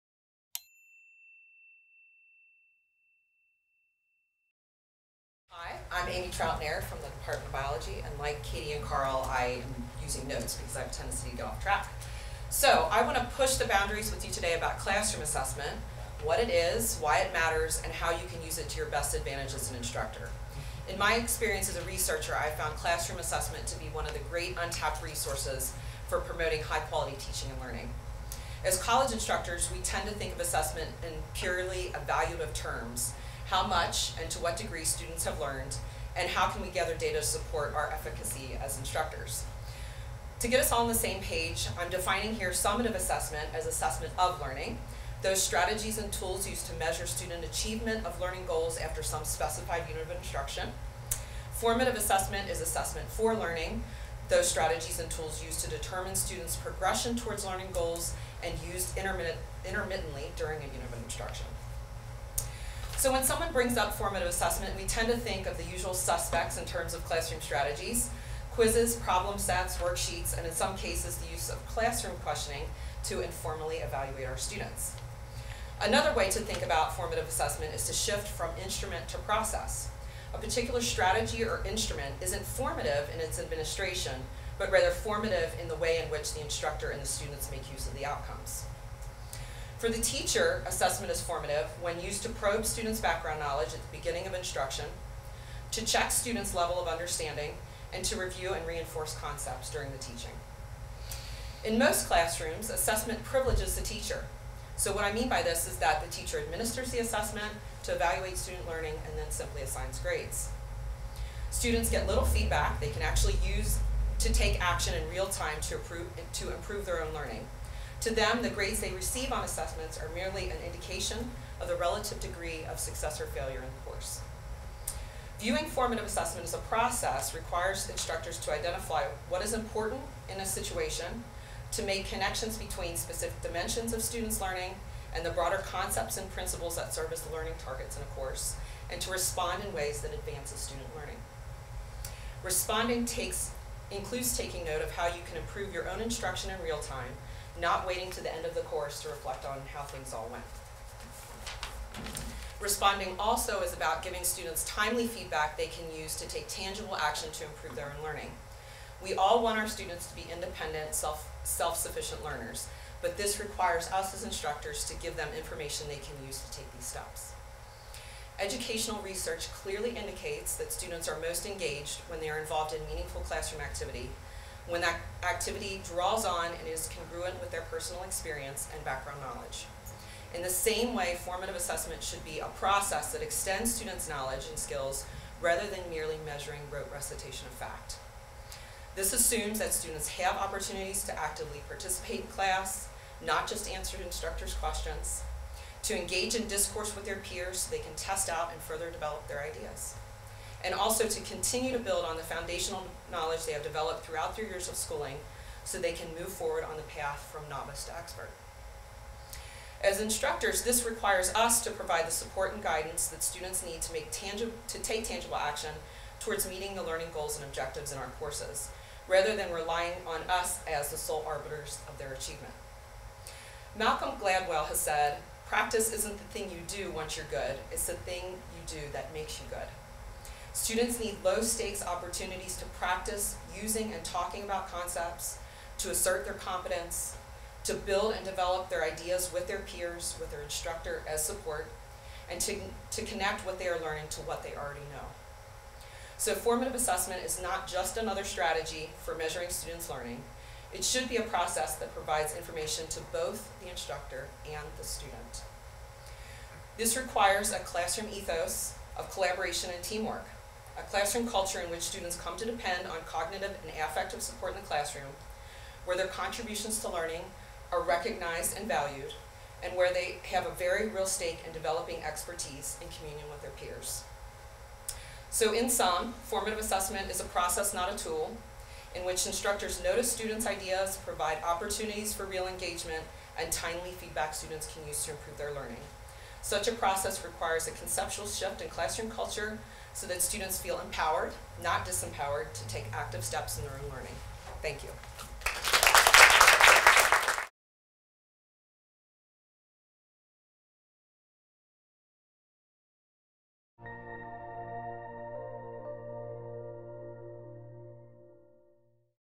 Formative Assessment: More Than a Tool for Measuring Learning
Many college instructors think of assessment as a set of tools or tests that they administer to students to measure mastery or achievement. While this is one function of assessment, it can be used in more sophisticated ways that elicit students' prior knowledge, their developing ideas, experiences, and values. Leveraging students' knowledge, ideas and experiences can be a powerful way to engage students in learning content matter and skills. One way to do this is formative assessment. Some educational researchers describe formative assessment as assessment for learning; that is, formative assessment is an set of instrumental and pedagogical strategies used to elicit student understanding at a particular point during instruction. This contrasts with summative assessment, or assessment of learning, which is any evaluation strategy such as multiple choice tests, research papers and essays that are used to measure students' achievement of course goals. I take these ideas of assessment one step further by asserting that classroom assessment, in particular formative assessment, is a tool for discursive engagement with students that is ongoing, relational and reflective. In this presentation I describe in more detail a relational philosophy of formative assessment, a standpoint that encompasses close listening and noticing of students' learning through their written and spoken discourse.

Amy Trauth-Nare is Assistant Professor of Biological Sciences. She earned her Ph.D. in Curriculum and Instruction at Indiana University Bloomington and her M.S. in Botany from Arizona State University. Amy teaches non-majors biology as well as science education courses required for elementary and middle level education majors. Her research focuses on classroom discourse and assessment, teacher and student identity, and environmental education. She regularly infuses the connections among society, technology and science into her teaching as a way to demystify the nature of science for students.